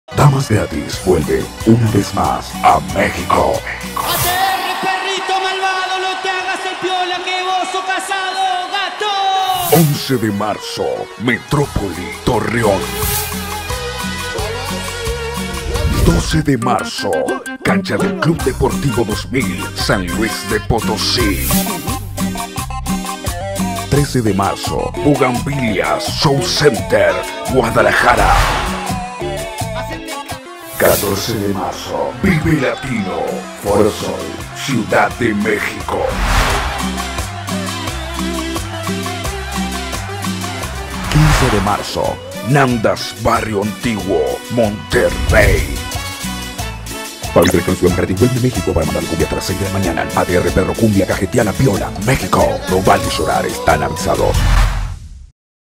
Damas Gratis - Gira Mexico Marzo 2020
11 Marzo - Metropoli (Torreon)
12 Marzo - Cancha de Club Deportivo 2000 (San Luis de Potosi)
13 Marzo - Bugambilias Show Center (Guadalajara)
14 Marzo - Vive Latino (Foro Sol, Ciudad de Mexico)
15 Marzo - Nandas (Barrio Antiguo, Monterrey)